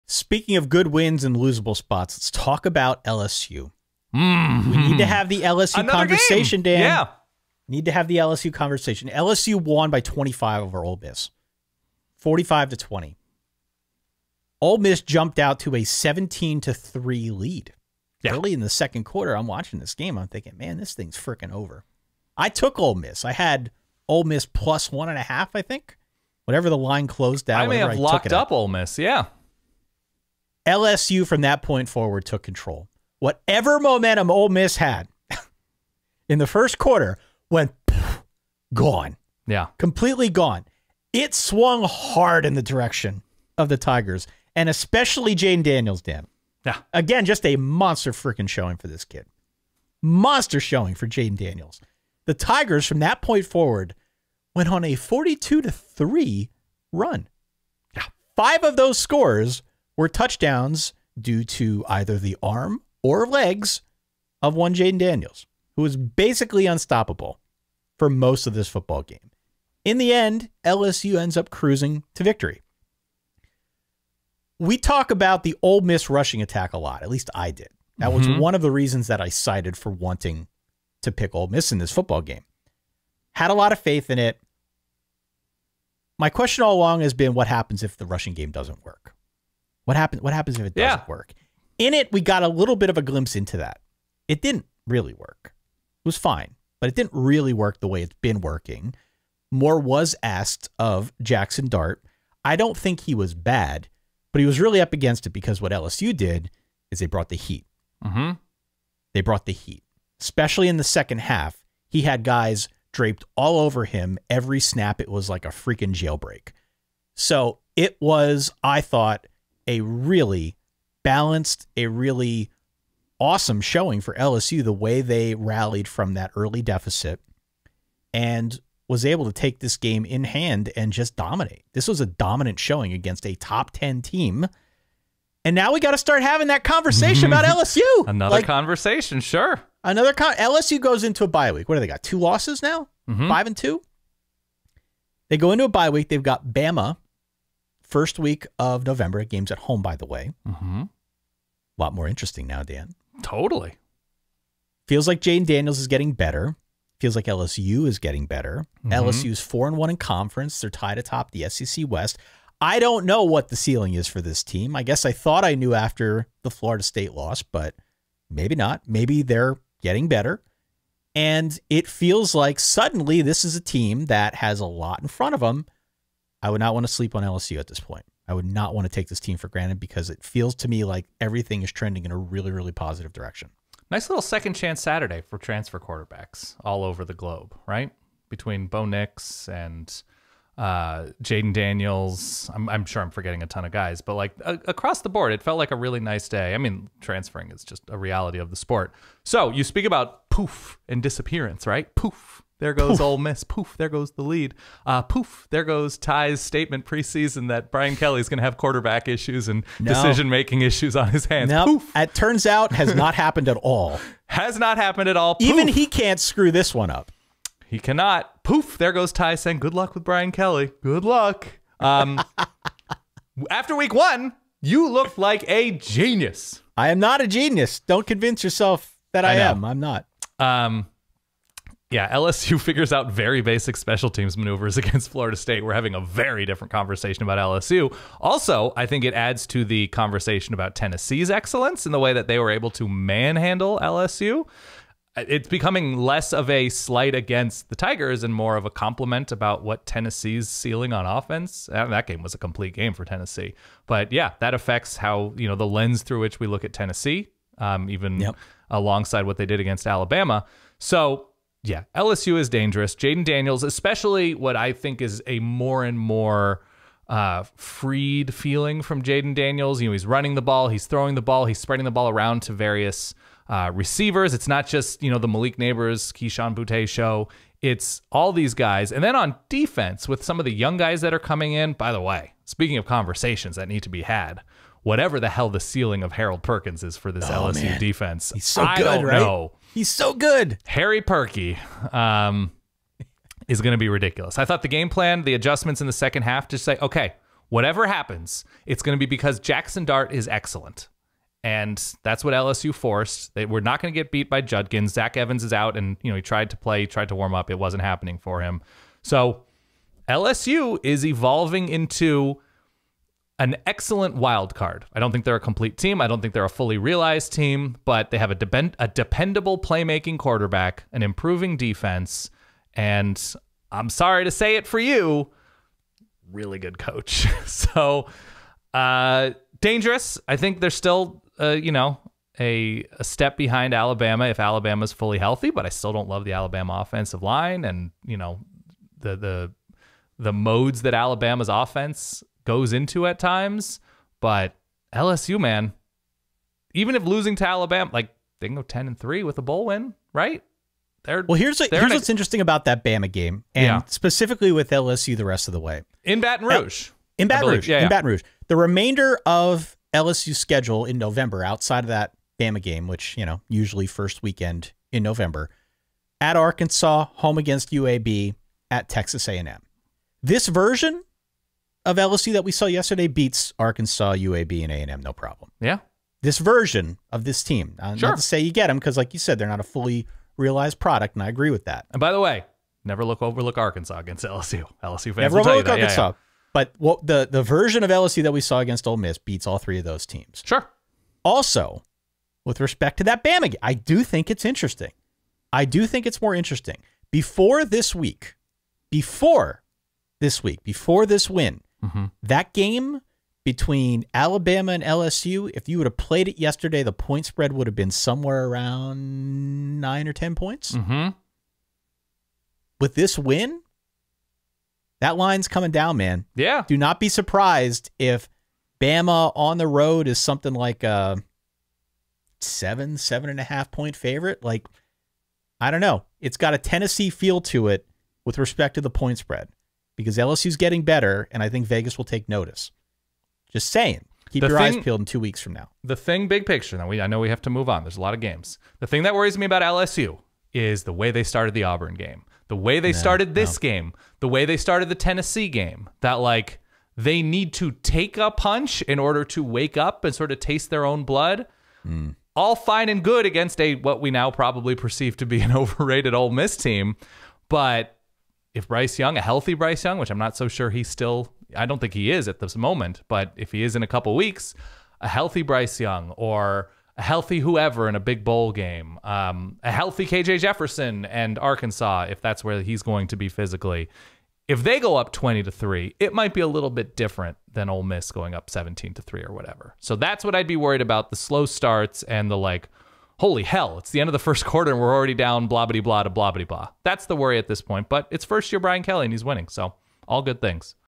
LSU CRUSHES OLE MISS - Are the Tigers the Real Deal?
The Solid Verbal College Football Podcast's reaction to LSU 45, OLE MISS 20. With LSU's emphatic comeback win over LSU, behind strong defensive adjustments and playmaking from guys like freshman LB Harold Perkins, as well as QB Jayden Daniels doing, well, everything, LSU came back from an early deficit against Lane Kiffin and Ole Miss, to put away their SEC West opponent. With the game now in the rear view mirror, what is LSU's revised ceiling with Nick Saban and Alabama on deck in two weeks?